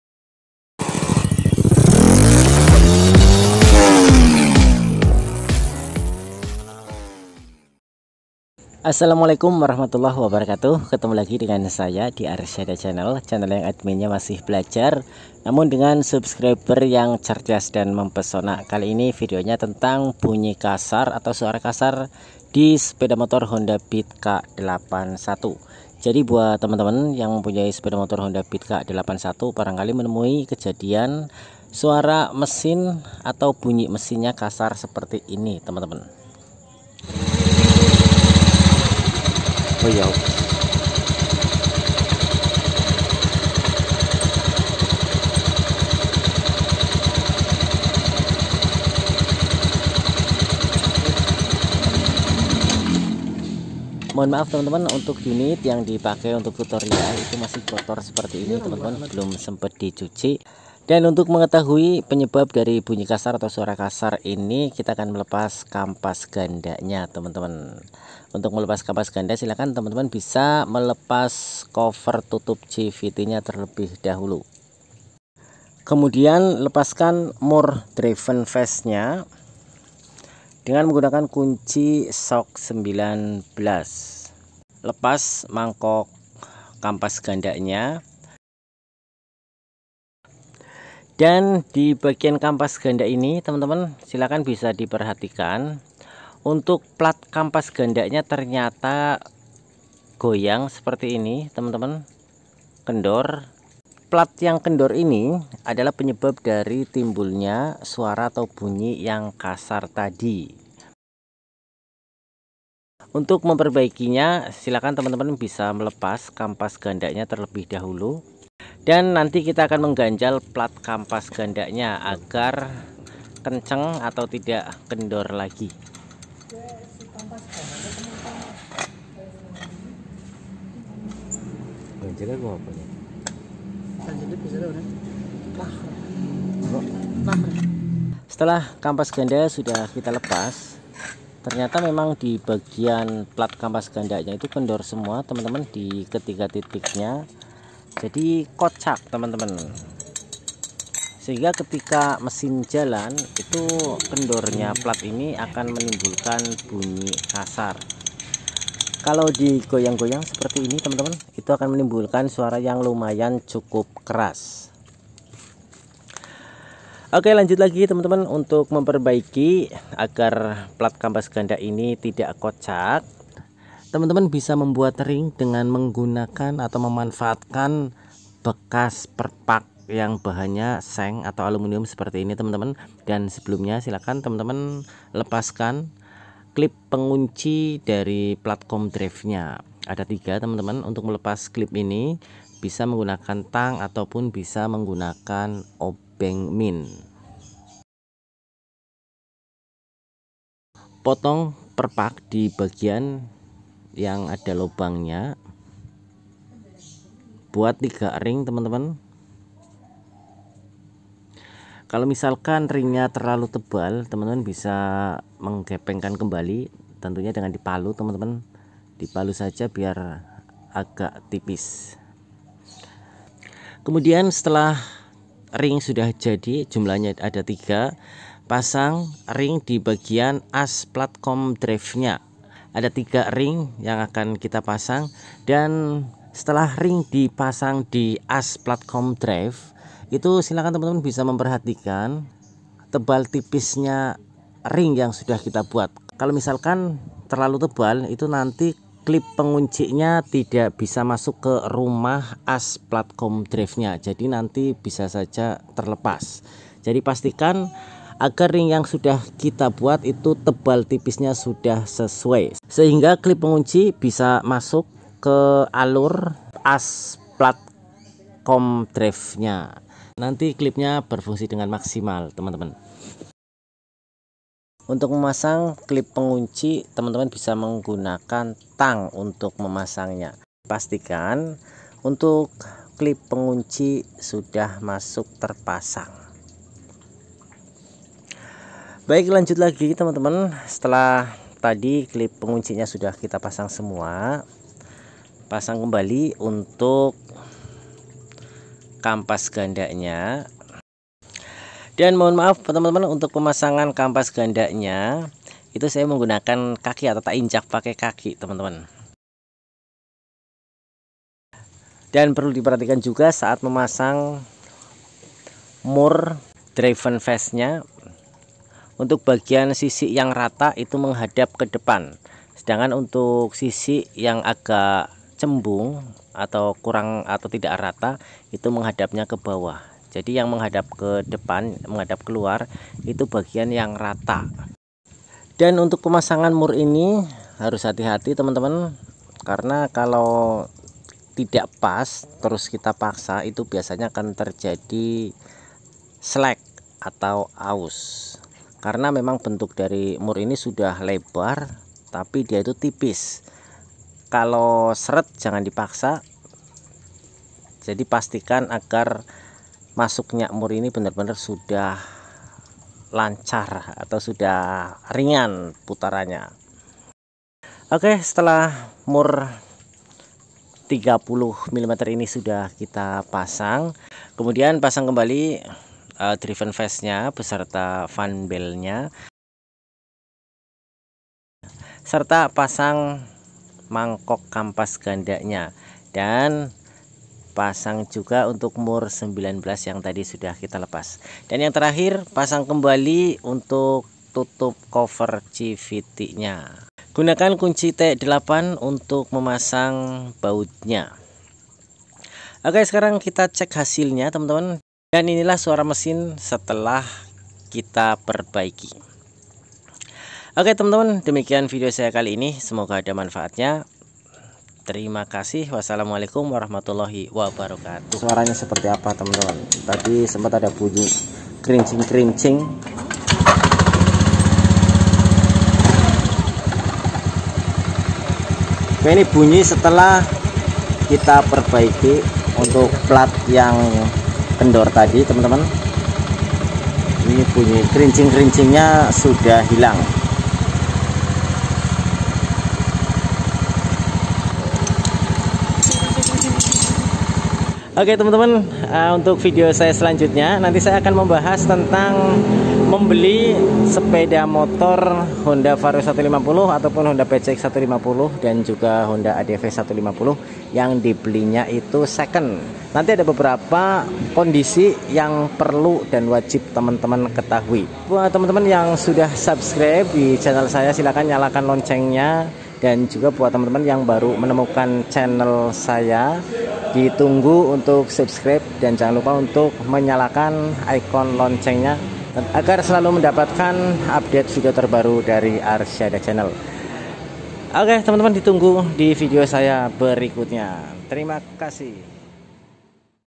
Penyebab Suara BERISIK Mesin Honda Beat
Video tentang perbaikan di sepeda motor honda beat yang bunyi atau suara Mesin honda beat kasar.
Gejala ini sering ditemui di honda beat 2019 atau honda beat k81.
Untuk honda scoopy terbaru dan honda vario 125 atau vario 150 serta beat terbaru 2020 ataupun honda scoopy terbaru belum ada kasusnya.
Semoga bermanfaat